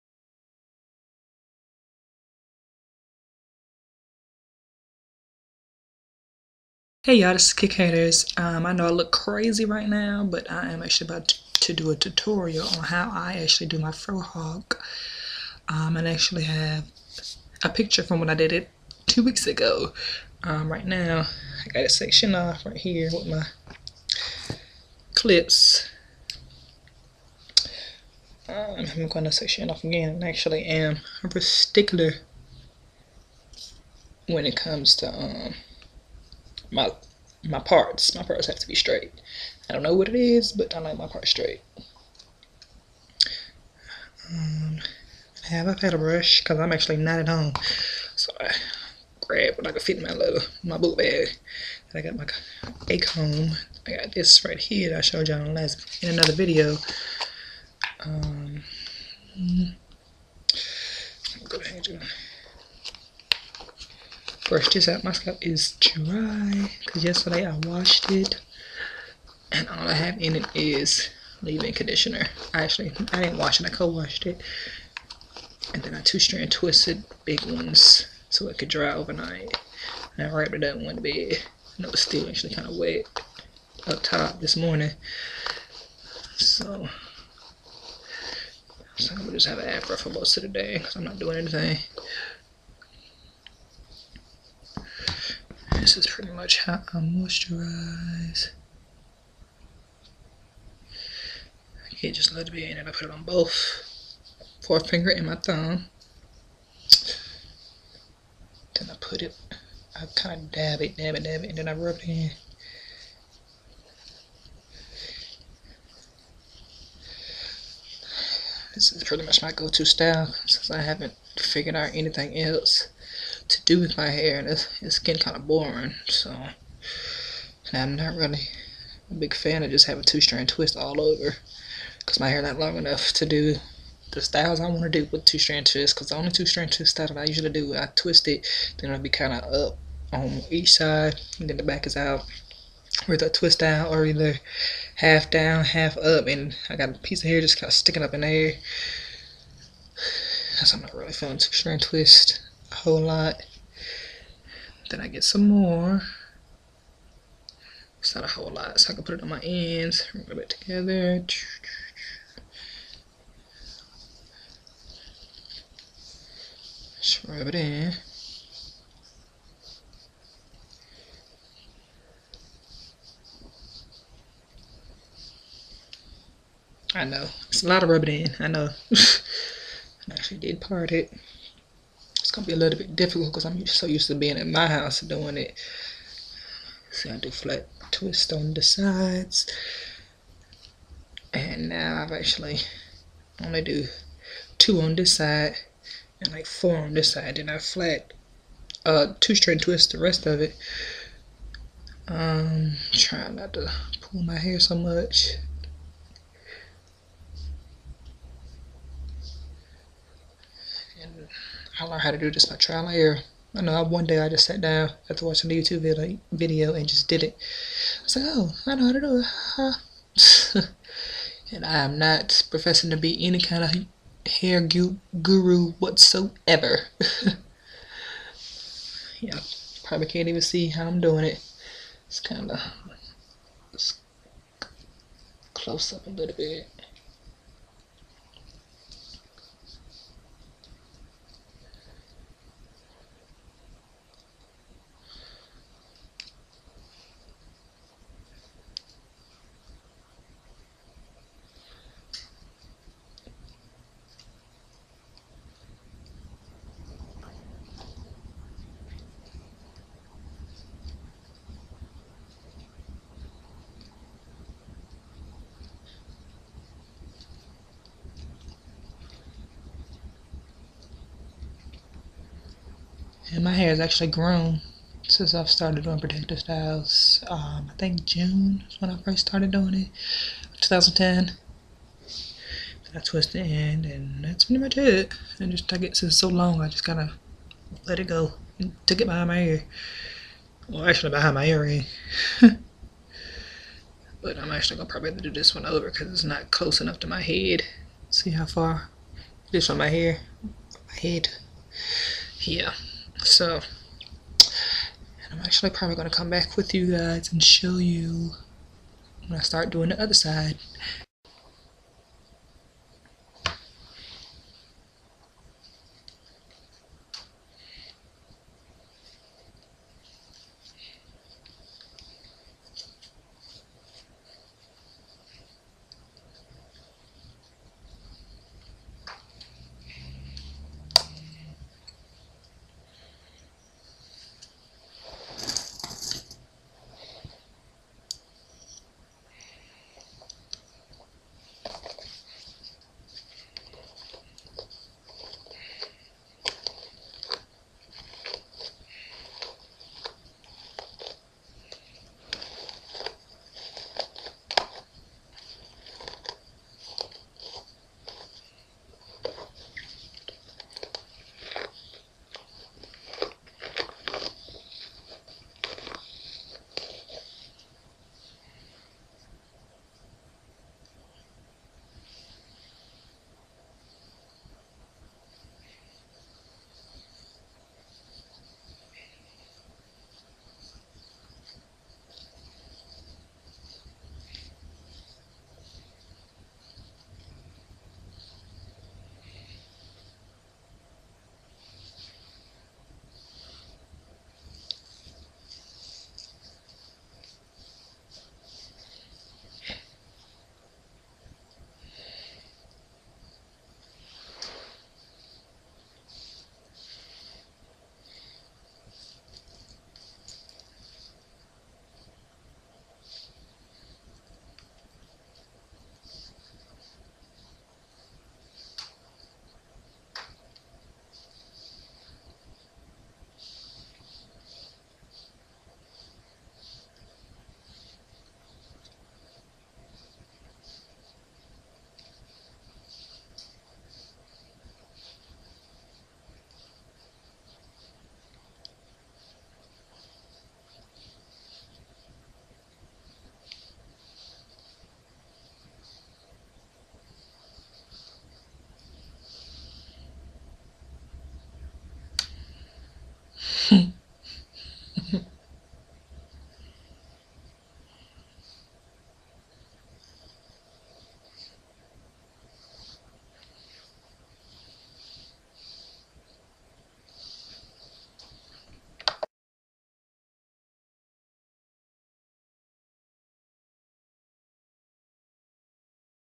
4C Natural Hair: Flat & Two Strand Twist Tutorial
Flat twisting the sides of my afro hair. My left side there are only two flat twists and my right side there are three. Each flat twist was parted using a rat-tail comb. The back and front of hair was two-strand twisted. The only products that were used were a leave in conditioner and Roots of Nature:Shea Butter Green Tea Triple Repair Hairdress.